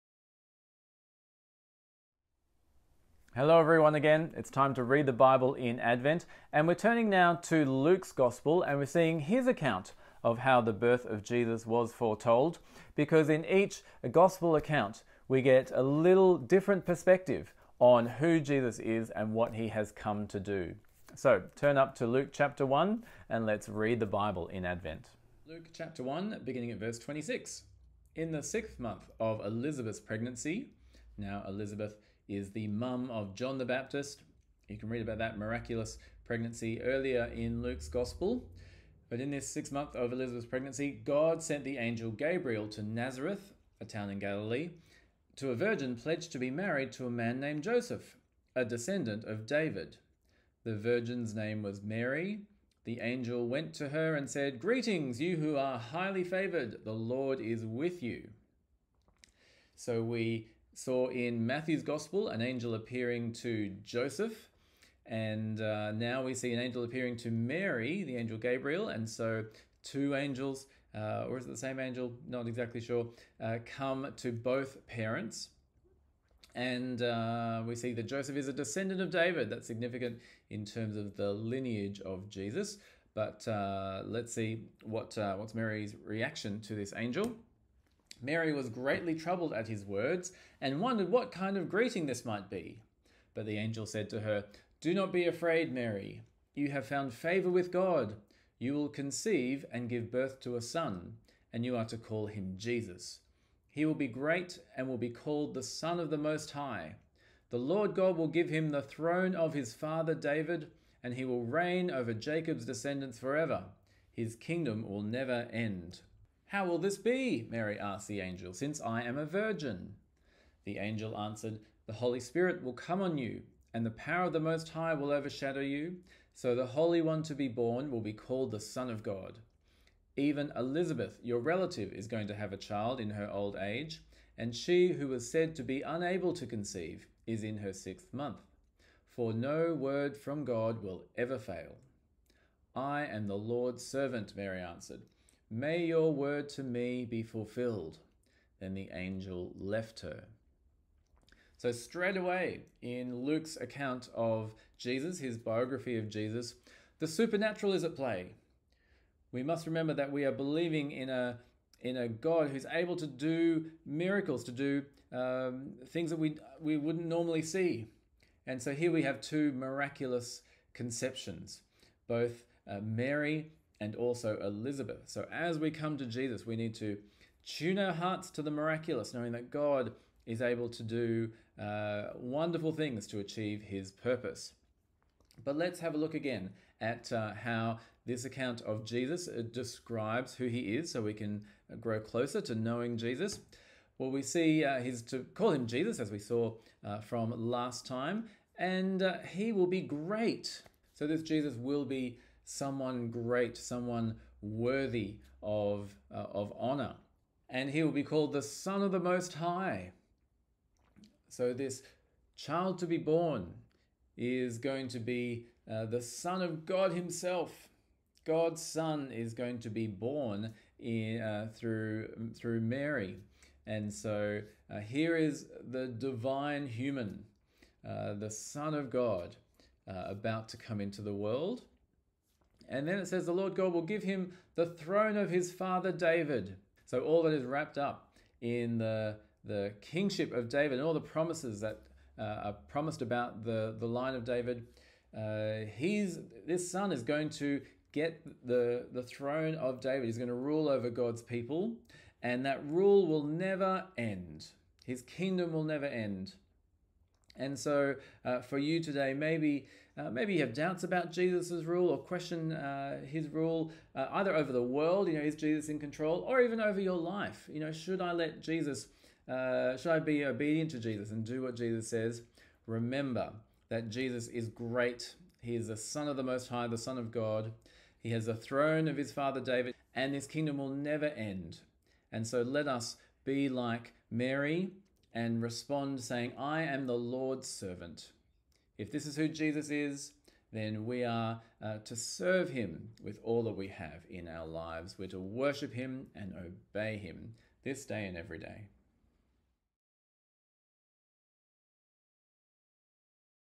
Read the Bible in Advent - Day 8 - Luke 1:26-38
As we turn to Luke's biography of Jesus, God is challenging us to trust the king that he has sent. Let's hear the word of the Lord today.